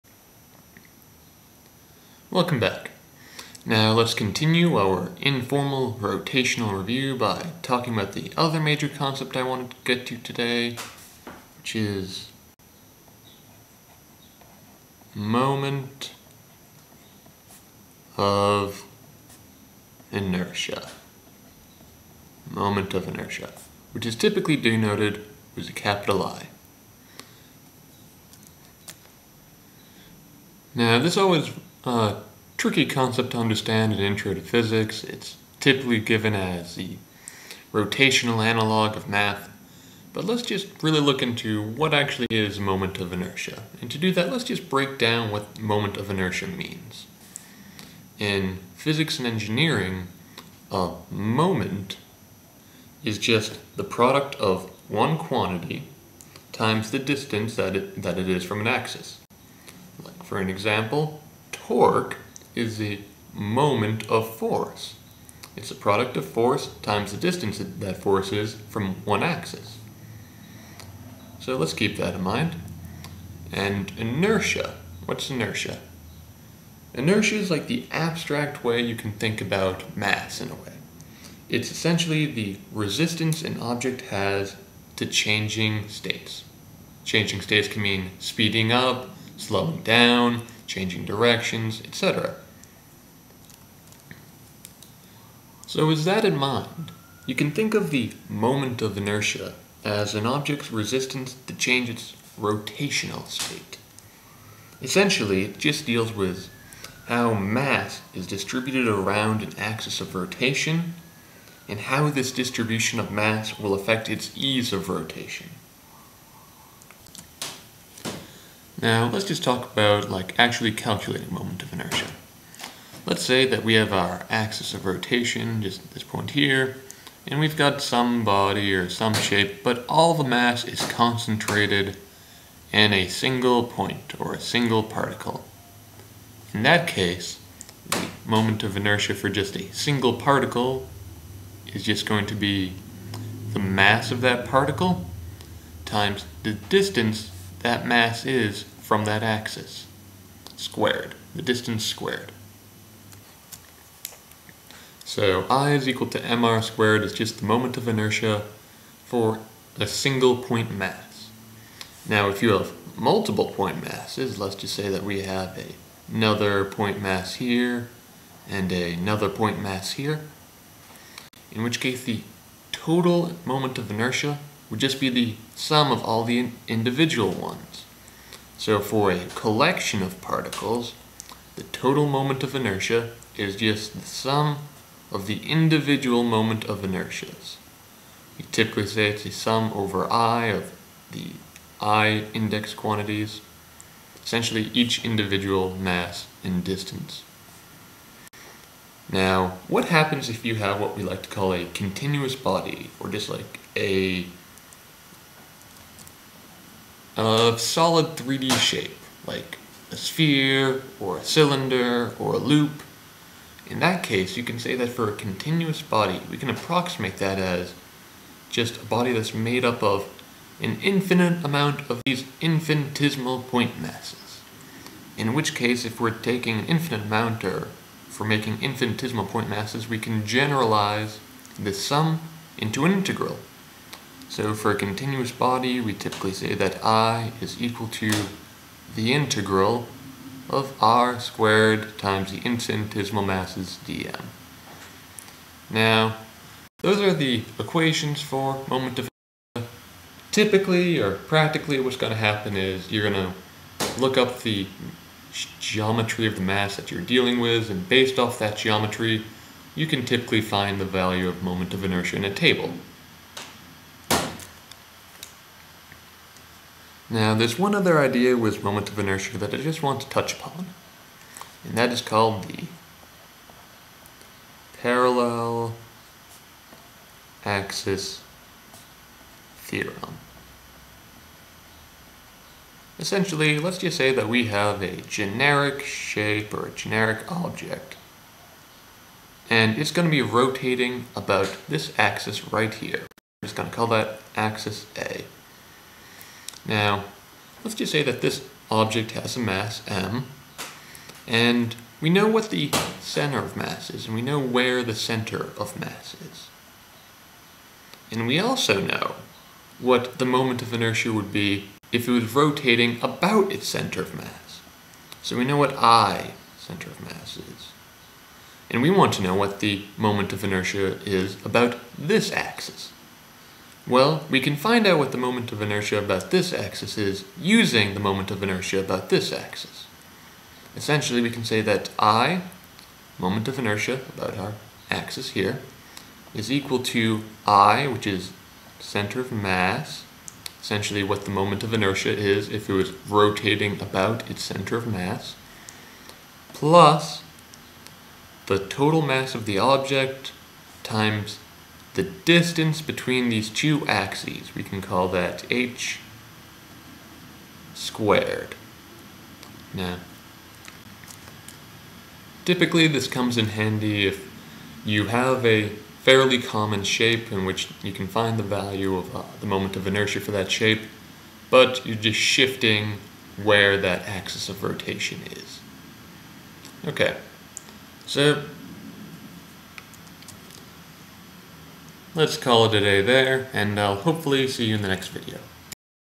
Moment of Inertia Review
A brief review of moment of inertia before we move onto pendulums